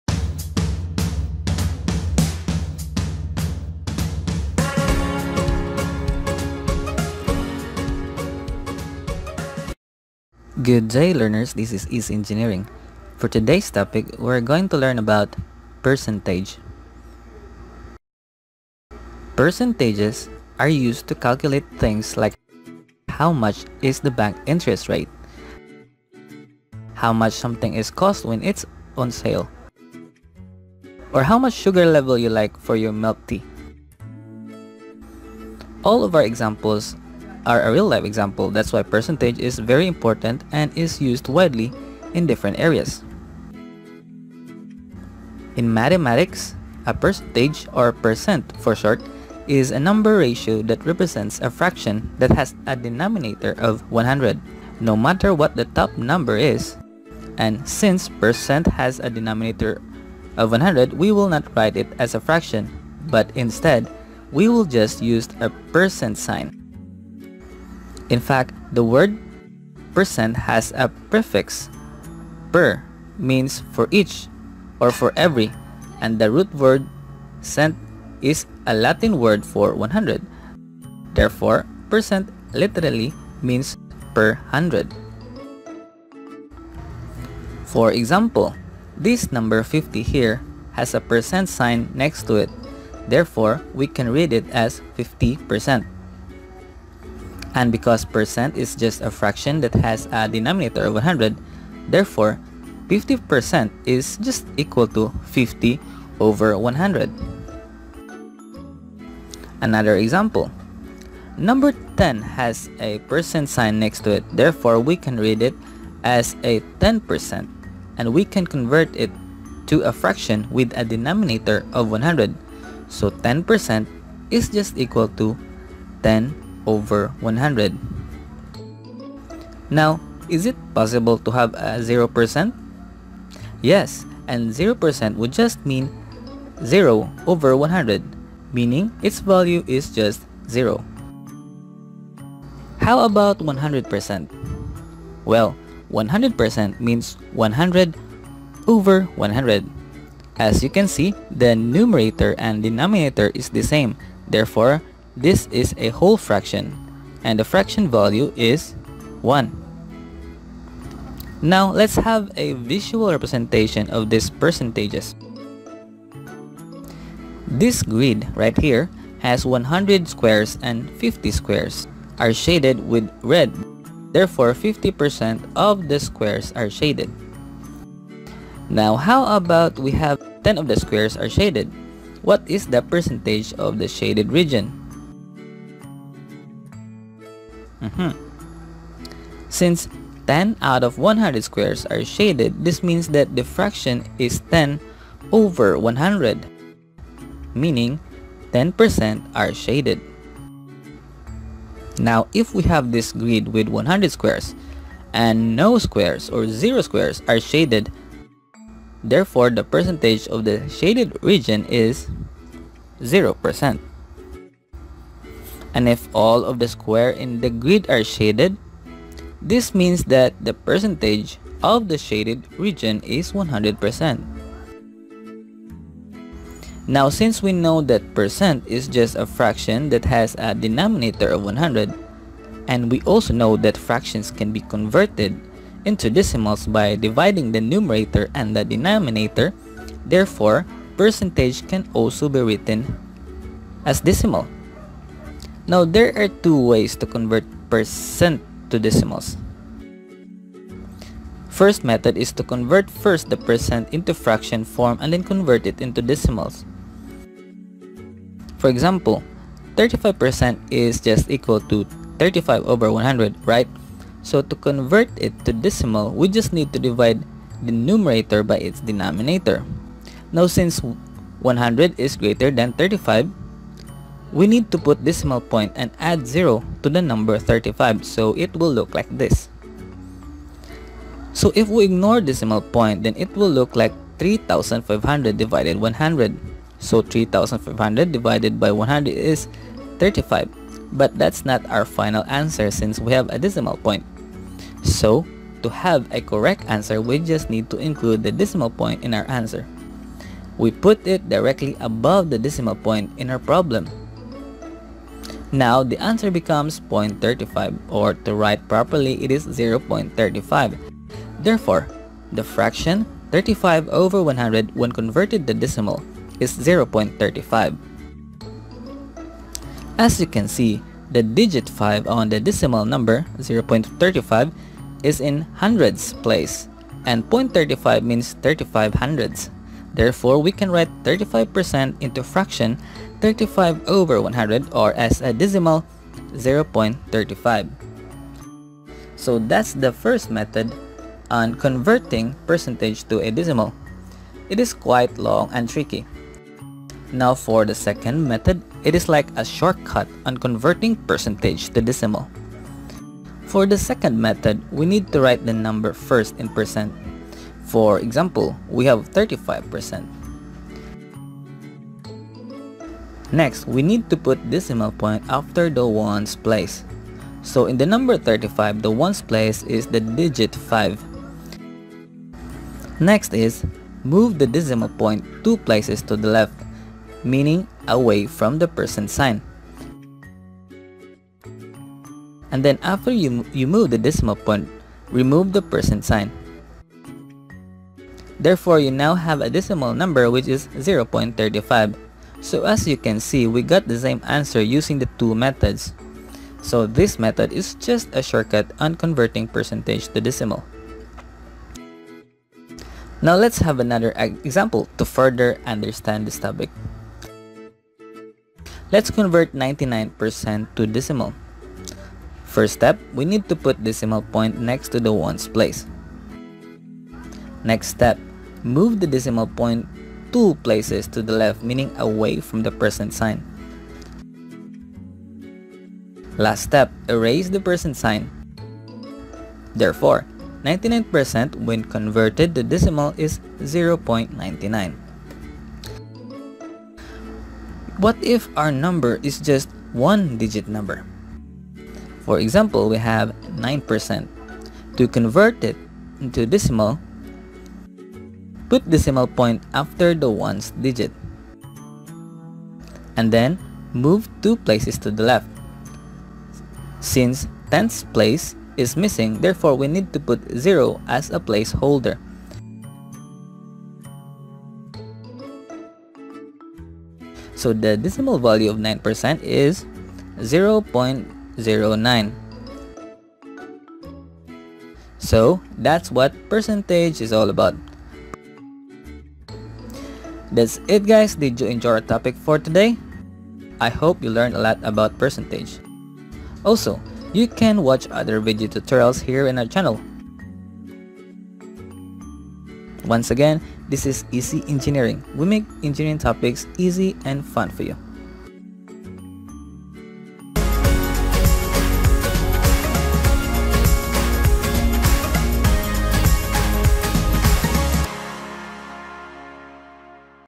PERCENTAGE | Math Animation
Percentage is used to calculate things like how much is the bank interest rate, how much something is cost when it is on sale, or how much sugar level you like for your milk tea. All of our examples are real-life examples that’s why the percentage is very important and is used widely in different areas.

In mathematics, a percentage or percent for short is a number ratio that represents a fraction that has a denominator of 100. No matter what the top number is, the bottom number will always be 100. And since percent has a denominator of 100, we will not write it as a fraction but instead, we will just use a percent sign (%) to represent percent. So whenever you will see a number that has this symbol (%) after the number, it means that the number is a percent or a fraction that has a denominator of 100. In fact, the word percent has a prefix per meaning “for each” or “for every” and the root word cent is a Latin word for 100, therefore percent literally means per hundred.

For example (flash example), this number 50 here has a percent sign next to it therefore we can read it as 50 percent (50%). And because percent is just a fraction that has a denominator of 100 therefore 50 percent is also equal to 50 over 100 (50/100).

*Enjoy this easy and fun video from EarthPen